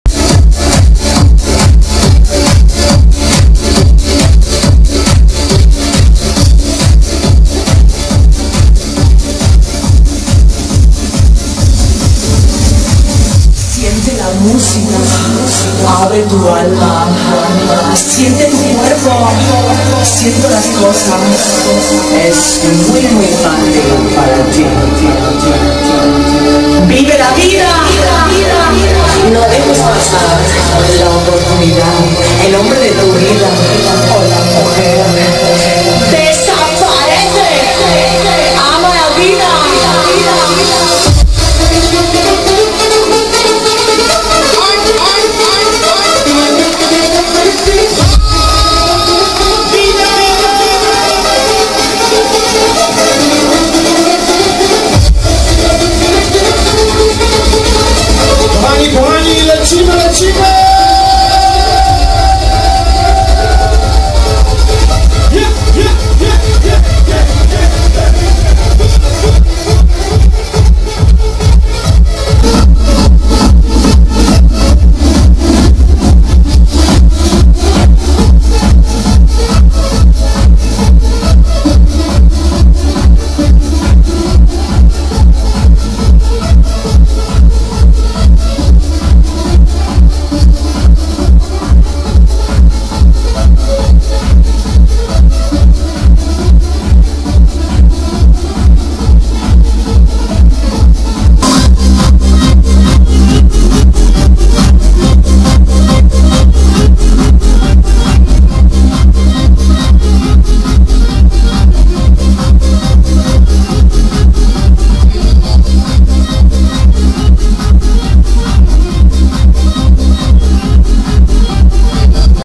Wesele Vixa Łódź cz.2 Manieczki
Ostra vixa pod koniec wesela :D
Manieczki Ekwador w Łodzi!
Lecimyyy świry :D